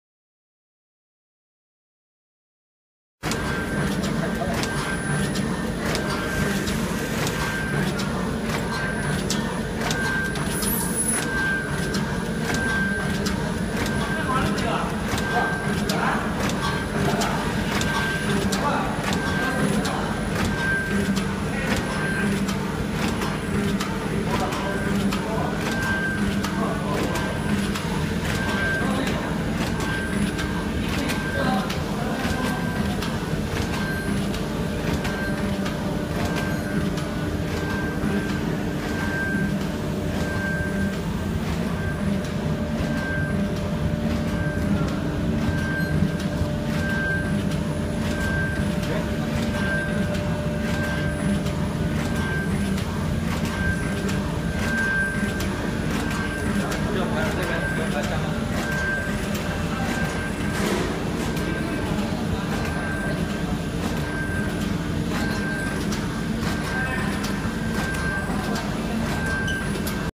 HFFS horizontal pouch packing machine. film roller pouch packing machine for granule and powder
CHANGZHOU QINCHENG PACKING MACHINERY CO;LTD

The machine using the international advanced technology and can be used for the trilateral and four edge-sealing aluminium film bag packaged products. the machine using microcomputer PLC programmable control, photoelectric sensor, using the servo motor.  flexible filling way, and micro particles of powder, water and dope different dosage forms, such as product choose different way to filling. This machine packing speed, wide measuring range, stable performance, convenient operation, good appearance is flat and level.

1.Capacity                40-80(bag/min)
2.volume                 100g
3.precision                ±1.5%
4.maximum bag            140(W)×180(H)mm
5. minimum bag            60(W)×70(H)mm
6.maximum film-roll diameter       Φ500mm
7.roll's interior diameter            Φ70-80mm
8.power                    3.5kw
9.voltage                    AC380V 50/60Hz
10.net weight                 850kg
11.dimension                 2800(L)×950(W)×2100(H)mm